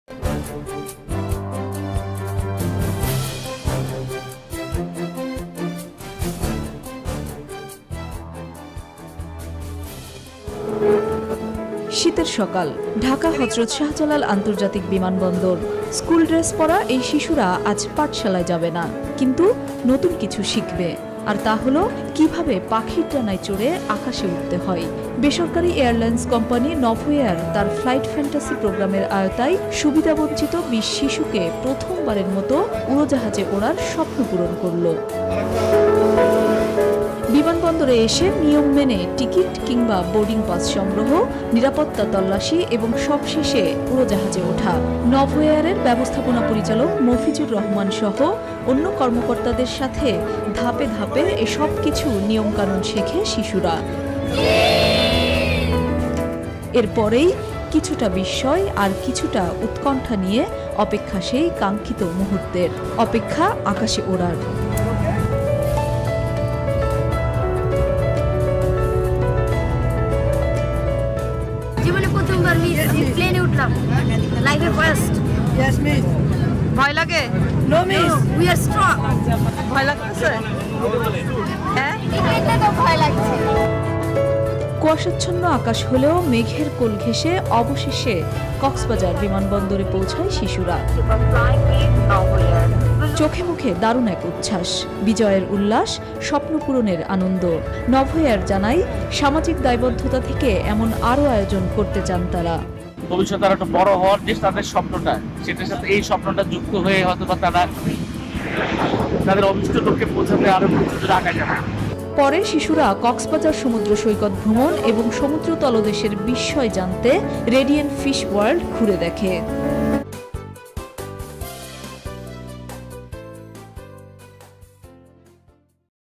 নভোএয়ারের মাধ্যমে তাদের সেই স্বপ্নপূরণ করেছেন_Channel Bangla News 24 TV- on You tube
থেকে ক্ষুদে শিক্ষার্থীরা যায় কক্সবাজার সমুদ্র সৈকত ও রেডিয়েন্ট ফিশ ওয়ার্ল্ড। সারাদিন পৃথিবীর দীর্ঘতম সমুদ্র সৈকতের অনিন্দ্য সুন্দর প্রকৃতি উপভোগ করে এই ক্ষুদে শিক্ষার্থীরা। পরে রেডিয়েন্ট ফিশ ওয়ার্ল্ডে আনন্দঘন সময় কাটায়। সেখানে বহু প্রজাতির সামুদ্রিক মাছ দেখে বিমোহিত হয় ক্ষুদে স্টাররা।